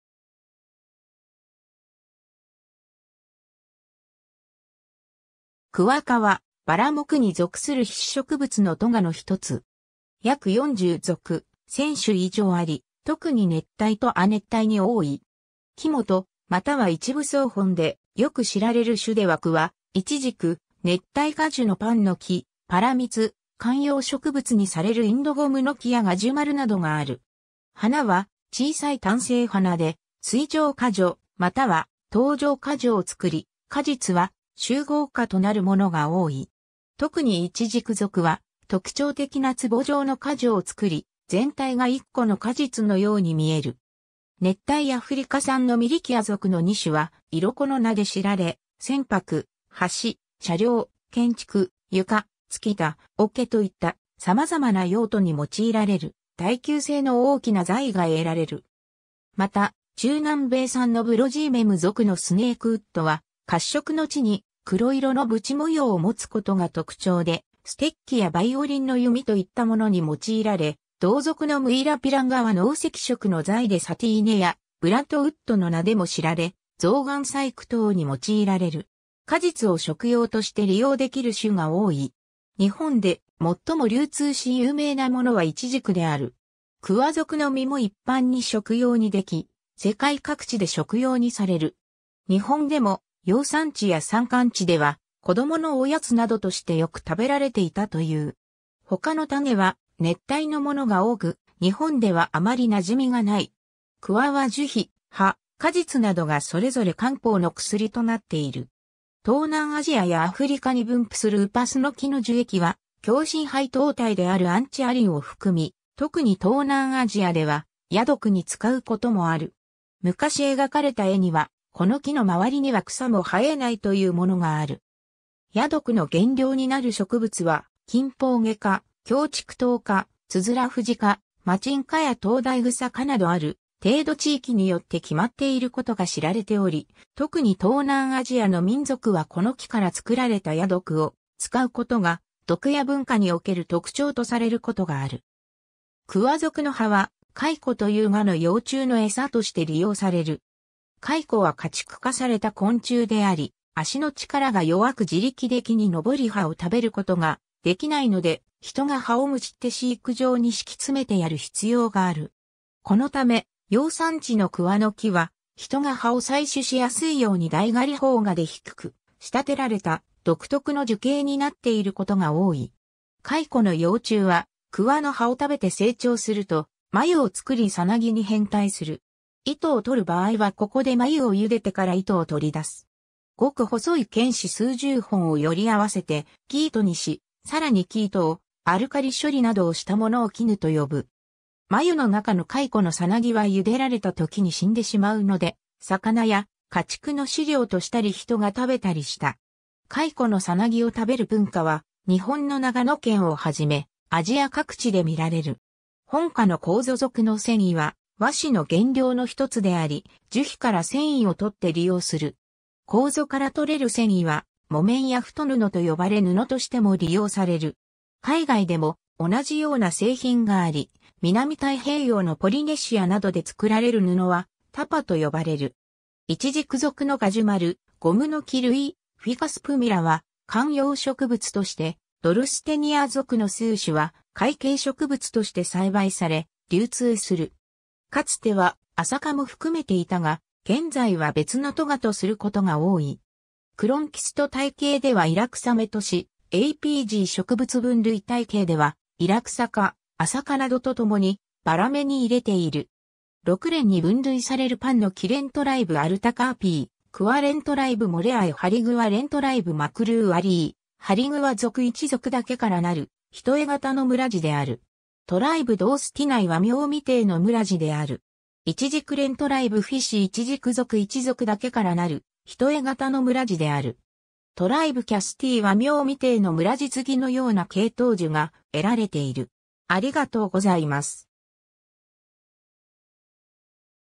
クワ科
クワ科 (学名 Moraceae) はバラ目に属する被子植物の科の一つ。
約40属、1000種以上（半分以上がイチジク属）あり、特に熱帯と亜熱帯に多い。
 木本または一部草本で、よく知られる種ではクワ、イチジク、熱帯果樹のパンノキ、パラミツ（ジャックフルーツ）、観葉植物にされるインドゴムノキやガジュマルなどがある。
花は小さい単性花で、穂状花序または頭状花序を作り、果実は集合果となるものが多い。
特にイチジク属は特徴的な壷状の花序（隠頭花序）を作り、全体が1個の果実のように見える。
 熱帯アフリカ産のミリキア属の2種（特にミリキア・エクスケルサ Milicia excelsa、シノニム: Chlorophora excelsa）はイロコの名で知られ、船舶・橋・車両・建築・床・つき板・桶といった様々な用途に用いられる耐久性の大きな材が得られる。
また中南米産のBrosimum属のスネークウッド（Brosimum guianense、シノニム: Piratinera guianensis）は褐色の地に黒色の斑模様を持つことが特徴でステッキやバイオリンの弓といったものに用いられ、同属のムイラピランガ（Brosimum rubescens、シノニム: B. paraense）は濃赤色の材でサティーネやブラッドウッドの名でも知られ、象嵌細工等に用いられる。
 果実を食用として利用できる種が多い。
日本で最も流通し有名なものはイチジクである。
クワ属の実も一般に食用にでき、世界各地で食用にされる。
日本でも養蚕地や山間地では子供のおやつなどとしてよく食べられていたという。
ほかの種は熱帯のものが多く日本ではあまりなじみがない。
 クワは樹皮、葉、果実などがそれぞれ漢方の薬となっている。
 東南アジアやアフリカに分布するウパスノキ（Antiaris toxicaria）の樹液は強心配糖体であるアンチアリンを含み、特に東南アジアでは矢毒に使うこともある。
昔描かれた絵にはこの機の周りには草も生えないというものがある。
矢毒の原料になる植物はキンポウゲ科、キョウチクトウ科、ツヅラフジ科、マチン科やトウダイグサ科などある程度地域によって決まっていることが知られており、特に東南アジアの民族はこの樹から作られた矢毒を使うことが毒矢文化における特徴とされることがある。
 クワ属の葉はカイコ（Bombyx mori）という蛾の幼虫の餌として利用される。
カイコは家畜化された昆虫であり、脚の力が弱く自力で木に登り葉を食べることができないので、人が葉を毟って飼育場に敷き詰めてやる必要がある。
このため養蚕地のクワの樹は人が葉を採取しやすいように台刈萌芽で低く仕立てられた独特の樹形になっていることが多い。
カイコの幼虫はクワの葉を食べて成長すると、繭を作り蛹に変態する。
糸を取る場合はここで繭を茹でてから糸を取り出す。
極細い繭糸数十本をより合わせて生糸にし、さらに生糸をアルカリ処理などをしたものを絹と呼ぶ。
繭の中のカイコの蛹は茹でられたときに死んでしまうので、魚や家畜の飼料としたり人が食べたりした。
カイコの蛹を食べる文化は日本の長野県をはじめ、アジア各地でみられる。
 本科のコウゾ属の繊維は和紙の原料の一つであり、樹皮から繊維を取って利用する。
コウゾから取れる繊維は木綿 (ゆう)や太布と呼ばれ布としても利用される。
海外でも同じような製品があり、南太平洋のポリネシアなどで作られる布はタパ（tapa）と呼ばれる。
 イチジク属のガジュマル、ゴムノキ類（ベンジャミン含む）、フィカス・プミラは観葉植物として、ドルステニア属（Dorstenia）の数種は塊茎植物（コーデックス）として栽培され、流通する。
 かつてはアサ科（アサ、ホップ、カナムグラなど）も含めていたが、現在は別の科とすることが多い。
クロンキスト体系ではイラクサ目とし、APG植物分類体系ではイラクサ科、アサ科などとともにバラ目に入れている。
 6連に分類される パンノキ連 tribe Artocarpeae クワ連 tribe Moreae ハリグワ連 tribe Maclureae ハリグワ属一属だけからなる単型の連である。
 tribe Dorstenieae 和名未定の連である。
 イチジク連 tribe Ficeae イチジク属一属だけからなる単型の連である。
 tribe Castilleae 和名未定の連 次のような系統樹が得られている。